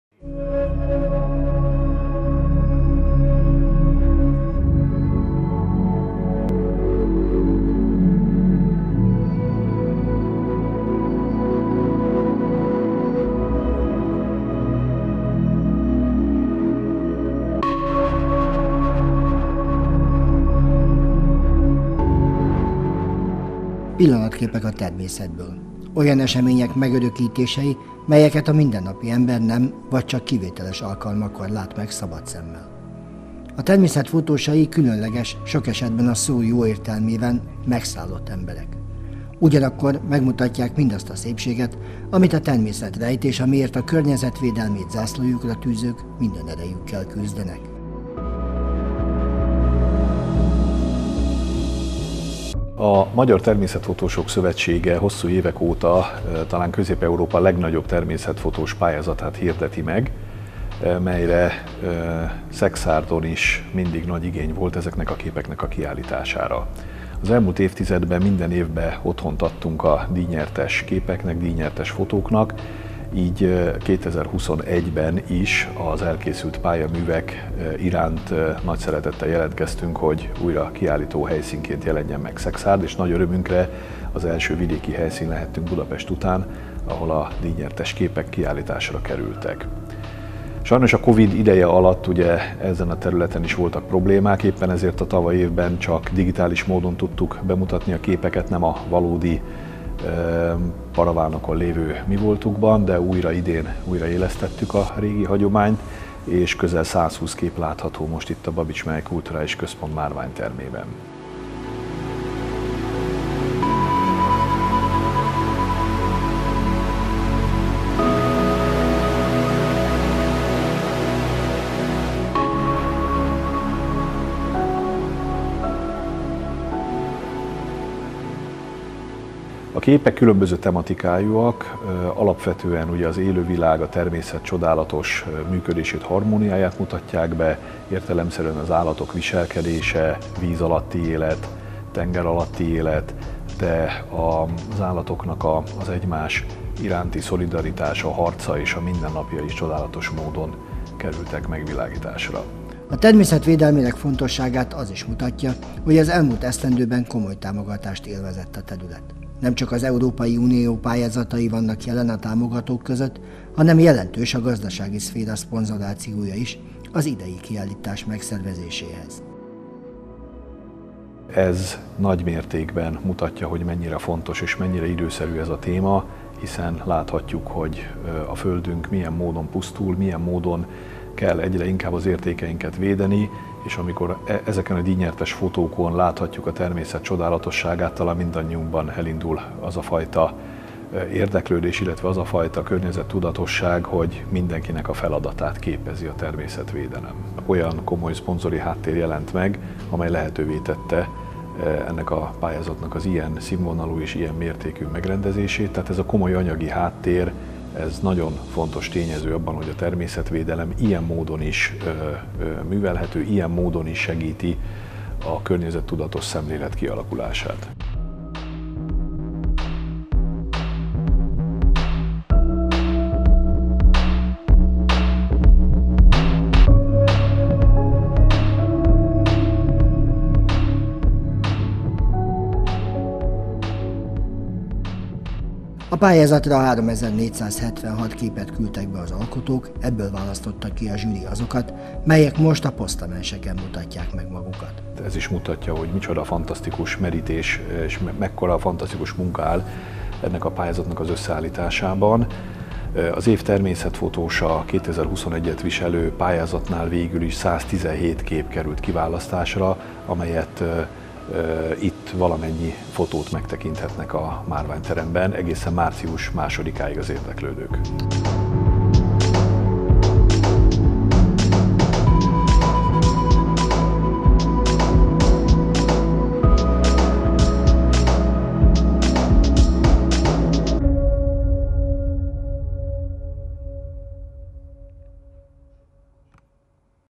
A természet képekben is csodálatos -fotókiállítás Szekszárdon a Babitsban
A természet szépségeit tárja a látogatók elé az idei, sorrendben 29. Természetfotó kiállítás,melynek a Babits Mihály Kulturális Központ adott otthont. A képek azokat a pillanatokat kapják lencsevégre, melyek a mindennapi ember számára rejtve maradnak a rohanó világban. A kiállítás a szervezők szándéka szerint a természetvédelem fontosságára is felhívja a figyelmet.